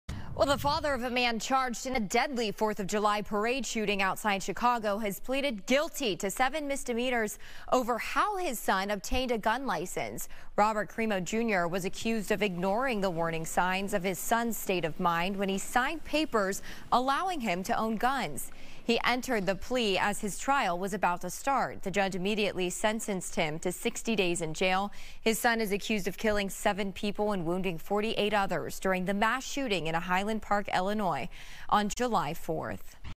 Father of accused Highland Park shooter pleads guilty
Robert Crimo Jr, the father of accused Highland Park shooter has plead guilty to seven misdemeanor charges over how his son obtained a gun license.